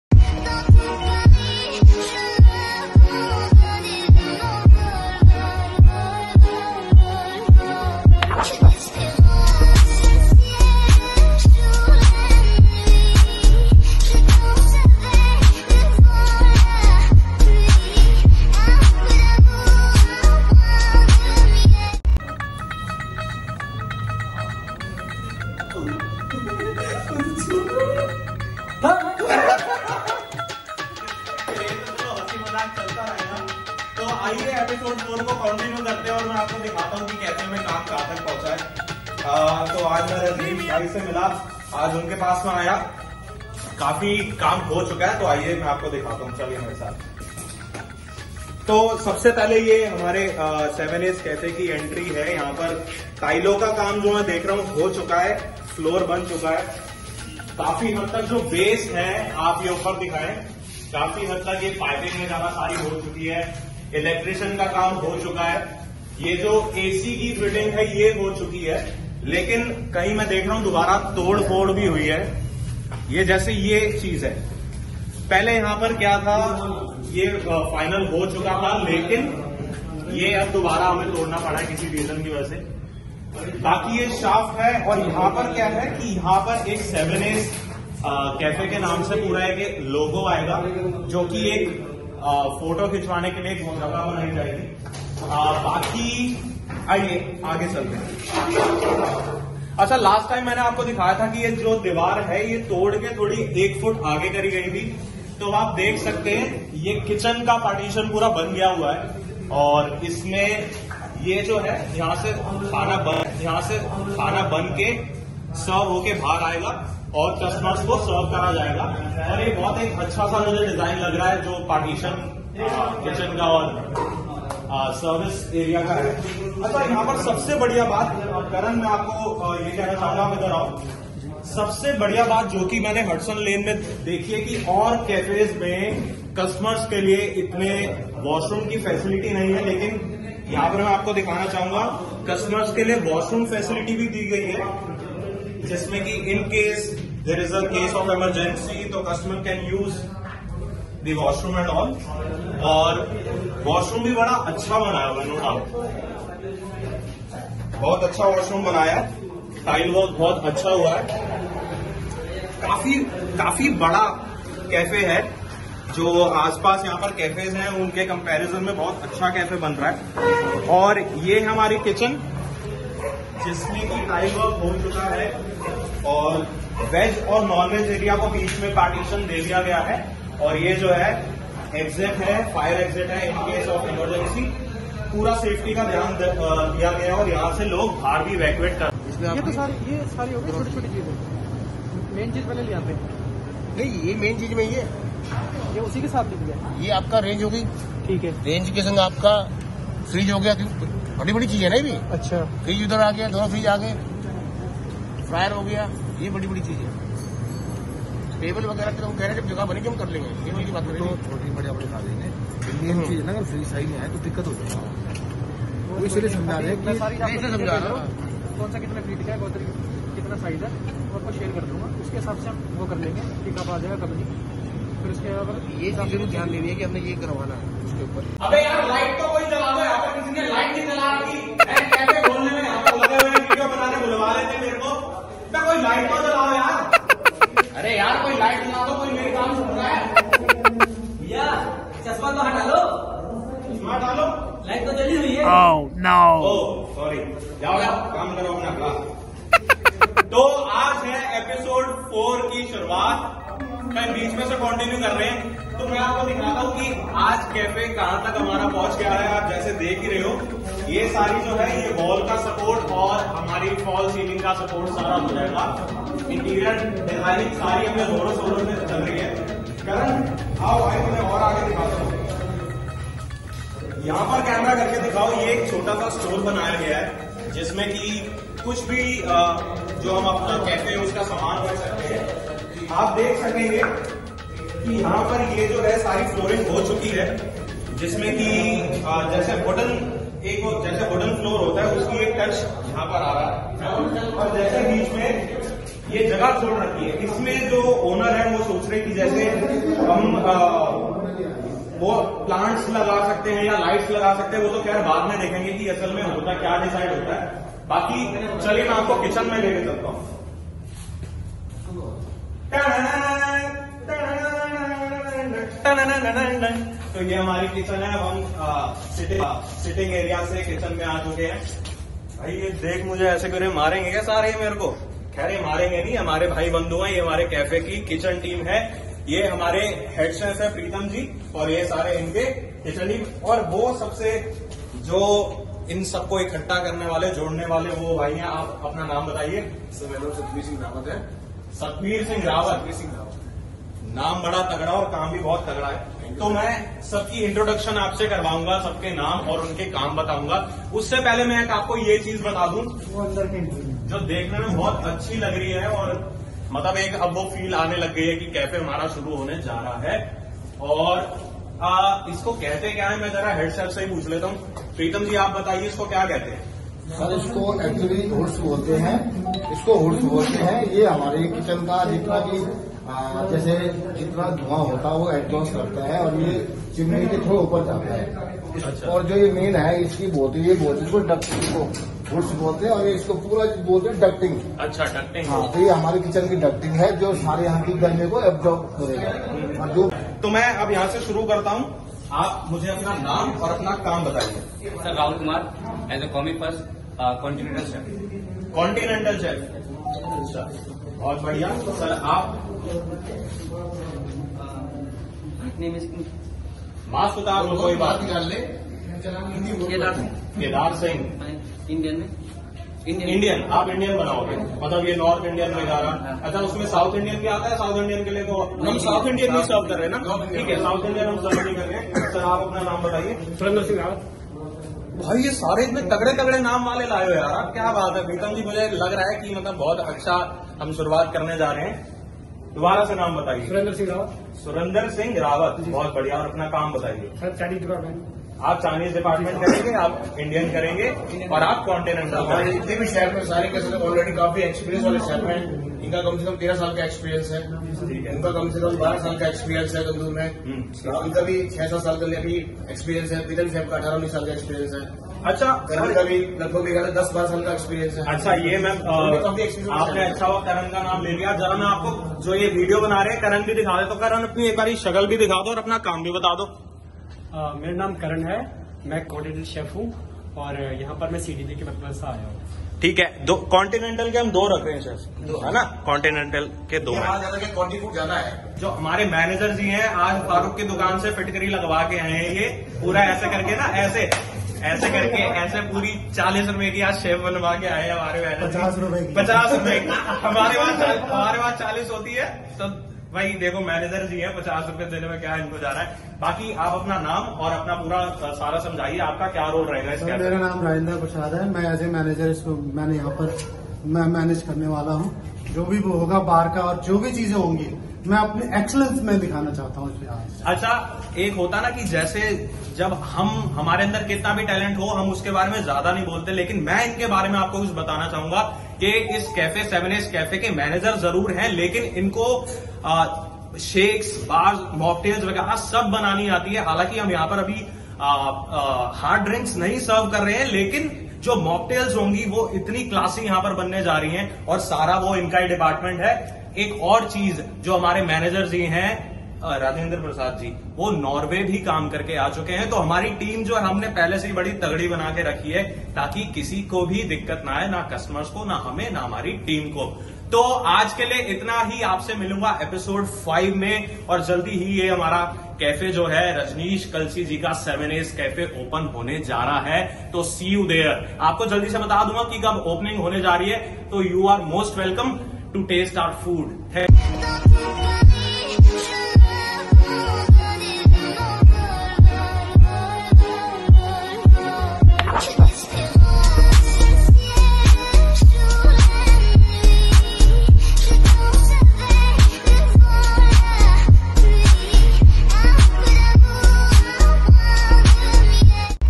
Episode 4 | The Kitchen Team is getting stronger | What does it take to open a cafe in Hudson lane?
It was amazing to meet the Continental, Indian and Chinese chefs!

The vibe is getting On!